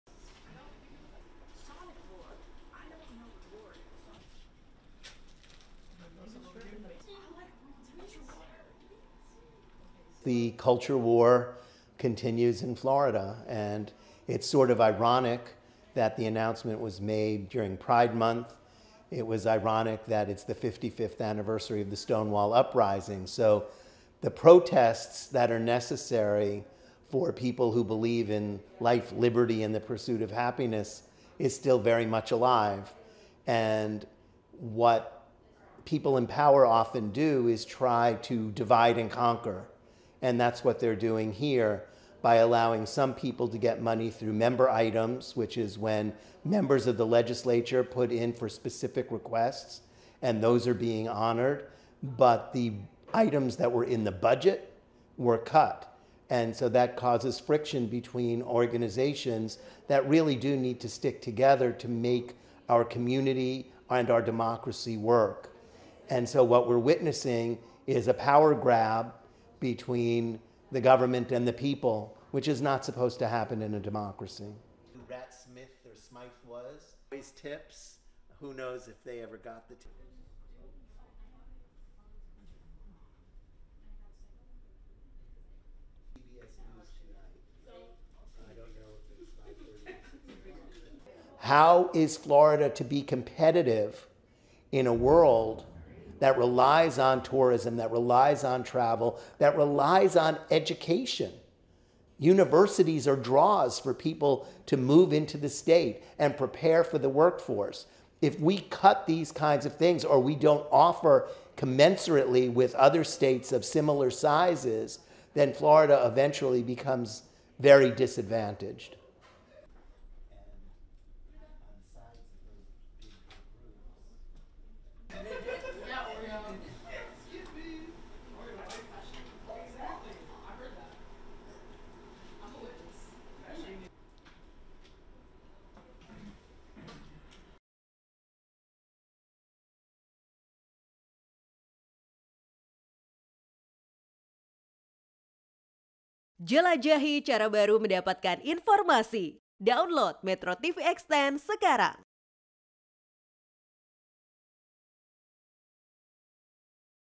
Florida Arts Groups Left In The Lurch By Desantis' Veto Of State Funding For Theaters And Museums
MetroTV,All Over Florida, Arts Groups Have Been Left Scrambling To Plug Large Budget Gaps After Florida Gov. Ron Desantis This Month Unexpectedly Vetoed $32 Million In Arts Funding.
Advocates Say The Lack Of State Funding Is Going To Devastate Arts And Culture In The Sunshine State.
Arts Leaders Across The State Say It’s The First Time They Recall A Governor Eliminating All Of Their Grant Funding.